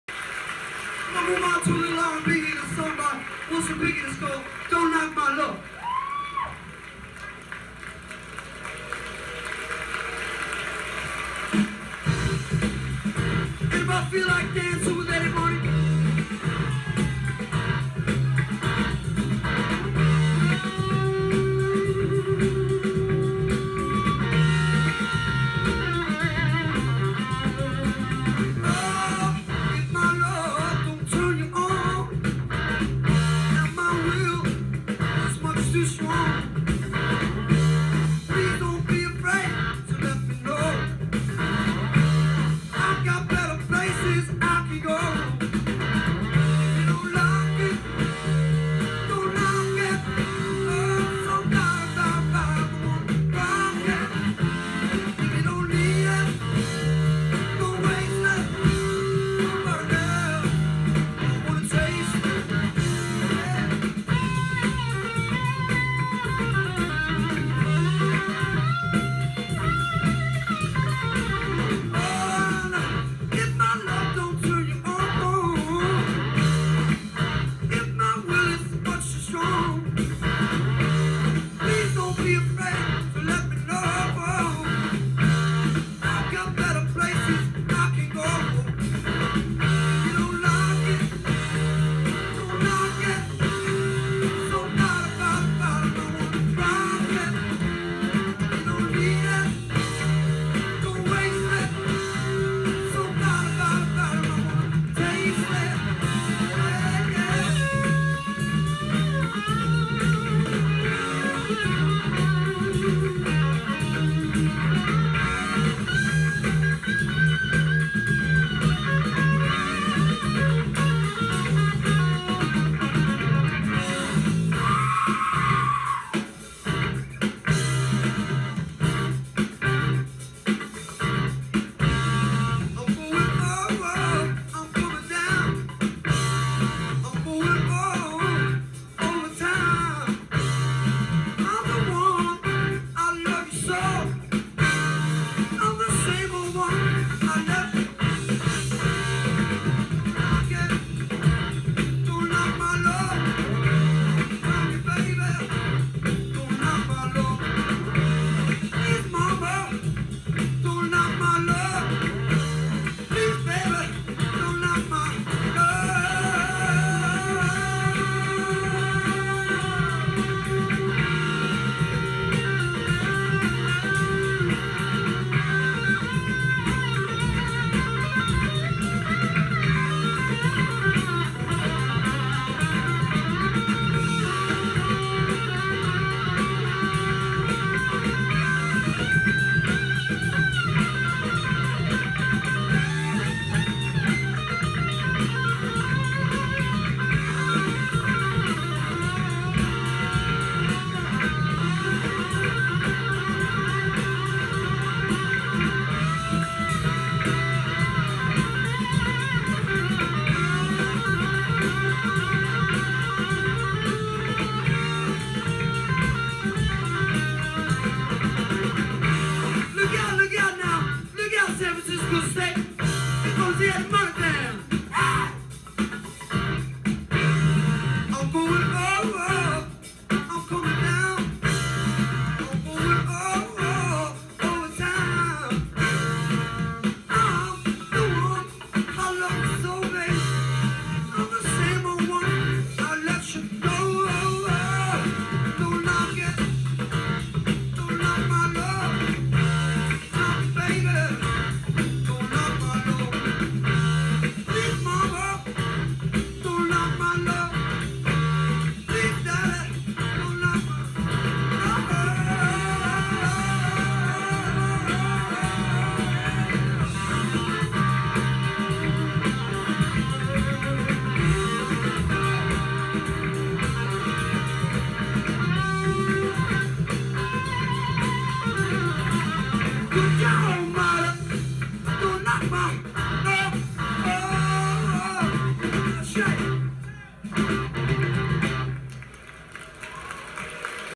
Eddie and Jimmy 1975
Early in their careers Eddie Money with Jimmy Lyons on guitar, San Francisco 1975.